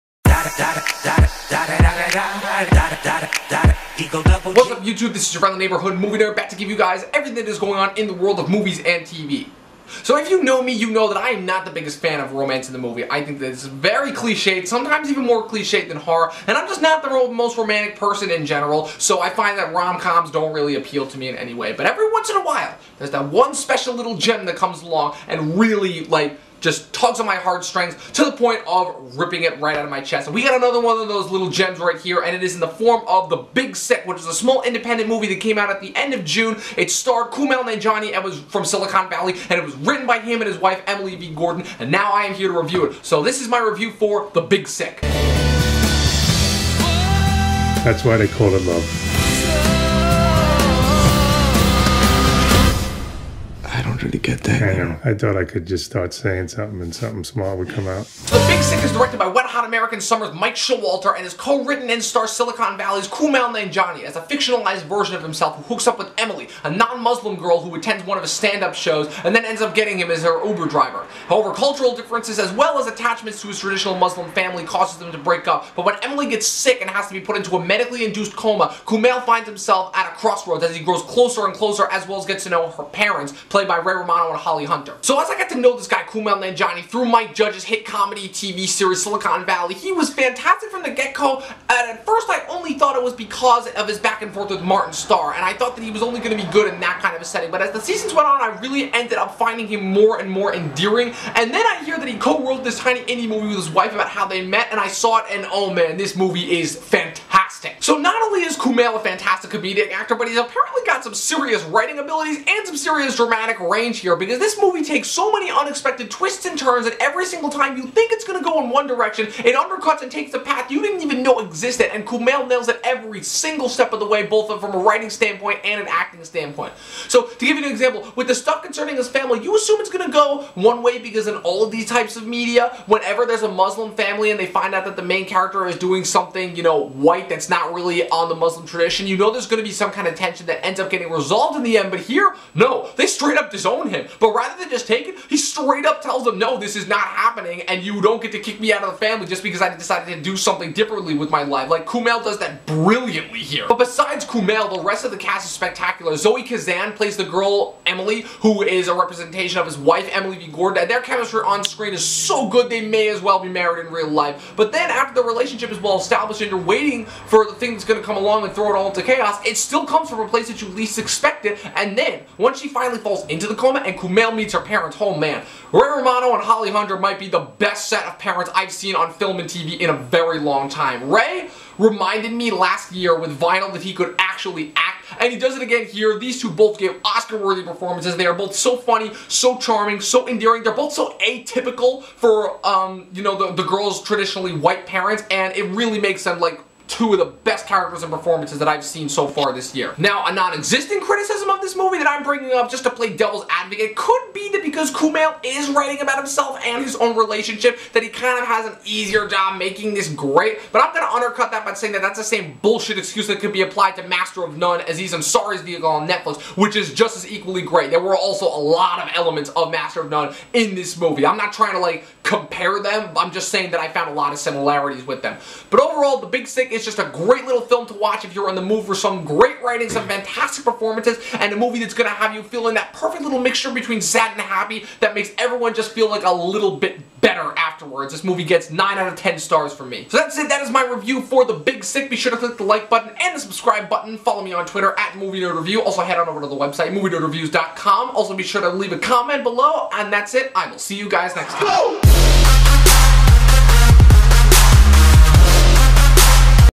The Big Sick Review
I'm keeping it coming with the Indies! This time, you've got Kumail Nanjiani from Silicon Valley writing this movie with his wife about how he met her, this is The Big Sick!

Also be sure to follow me on Stardust: MovieNerd